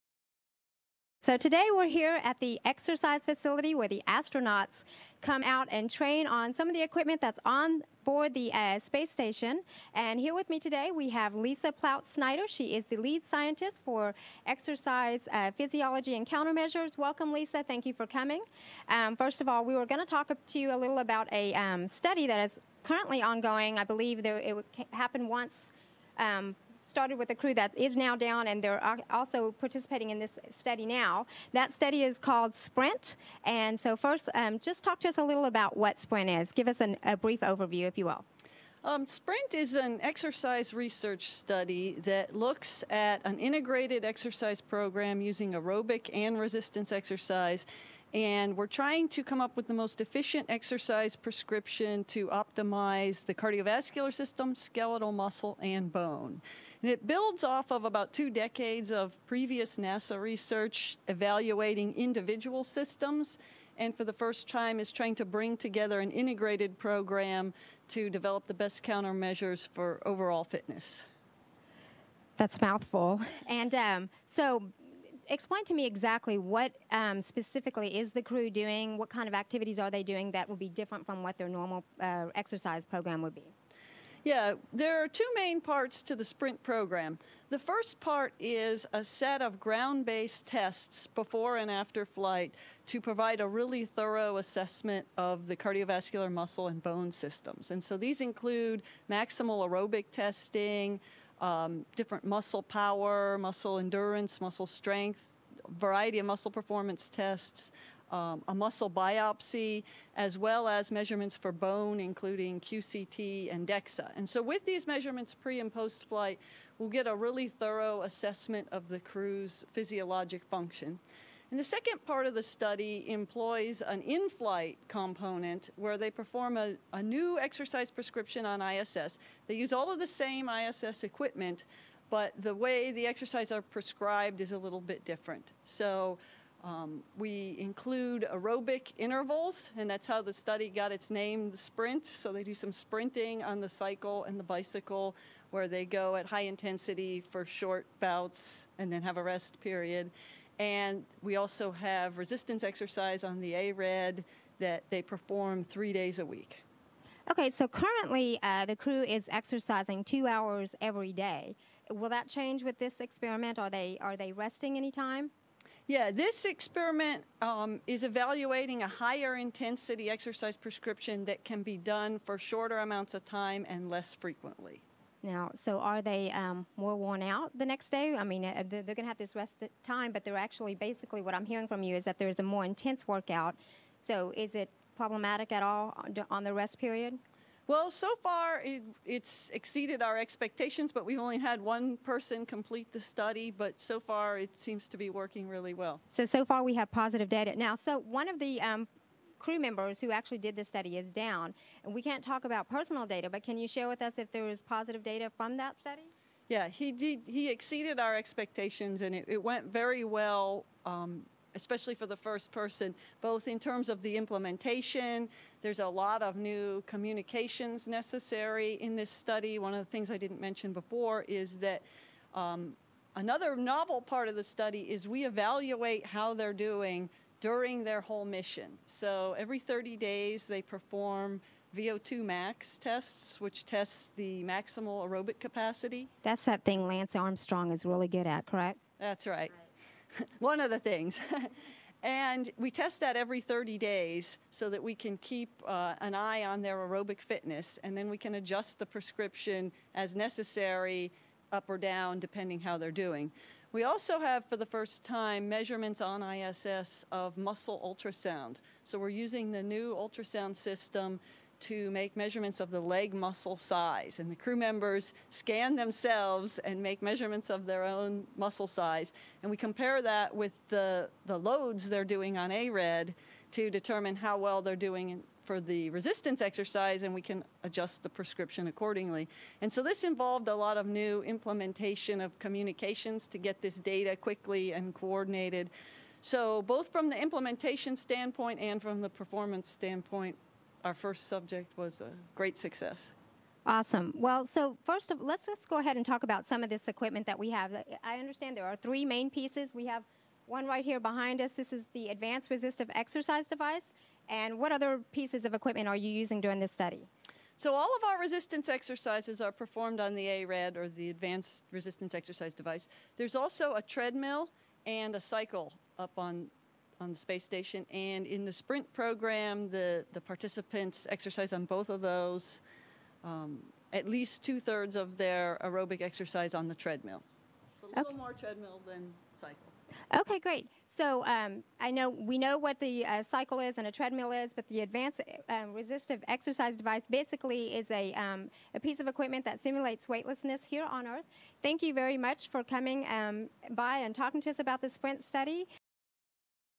Interview with Lori Ploutz-Snyder, Ph.D. about SPRINT
An interview with NASA Lead Exercise Physiology Scientist Lori Ploutz-Snyder, Ph.D. about the SPRINT exercise program used by the crew members aboard the International Space Station.